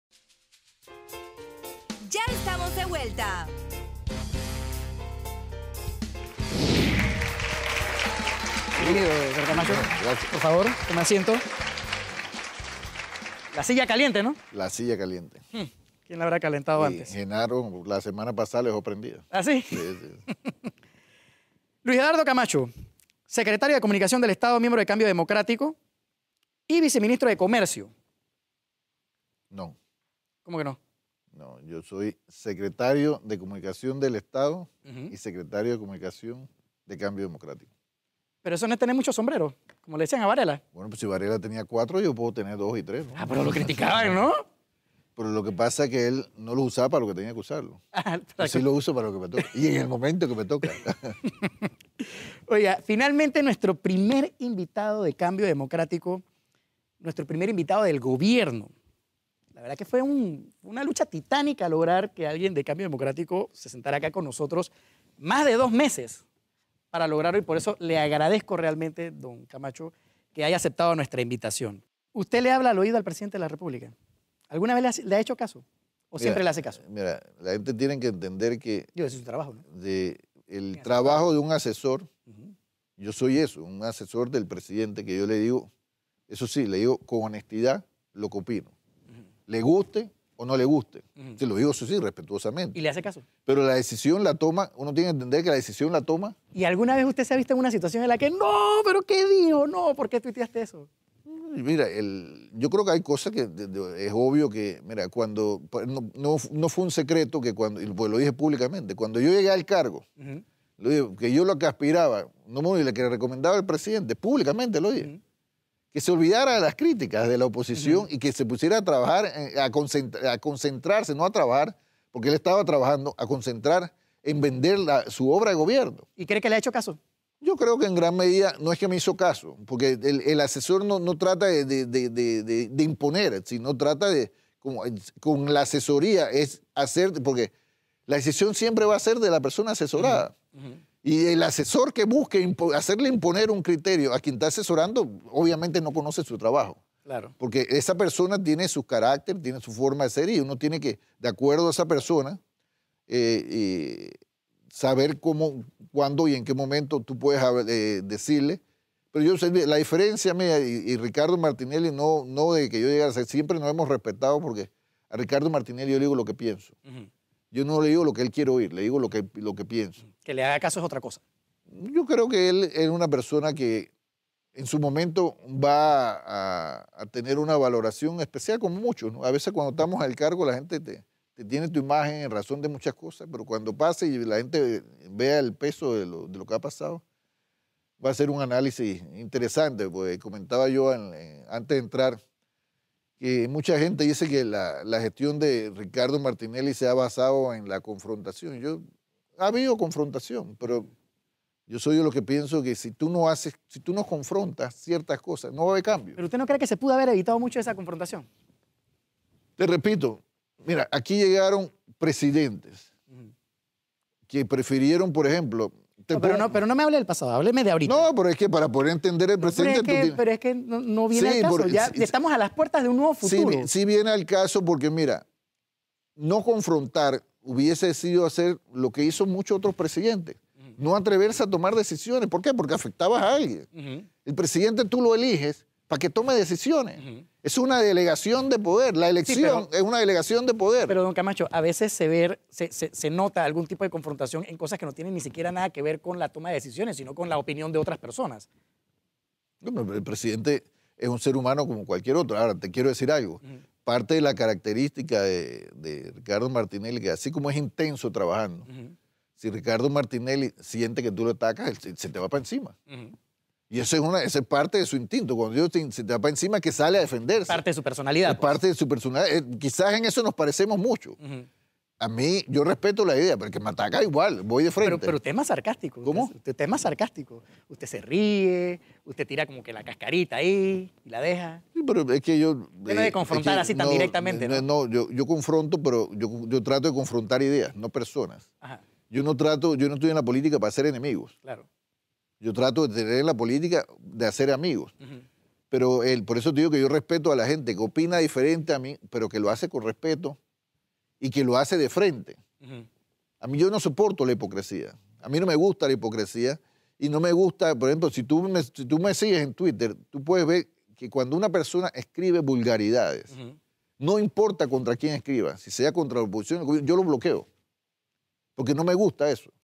ENTREVISTA A LUIS EDUARDO CAMACHO - PARTE 1
El Secretario de Comunicación del Estado Luis Eduardo Camacho contestó todas las preguntas que le formuló Eric Martinez (@emartineztv). Hay opiniones encontradas sobre si contestó guabinosamente o con la verdad, pero contestó. Aquí la primera parte.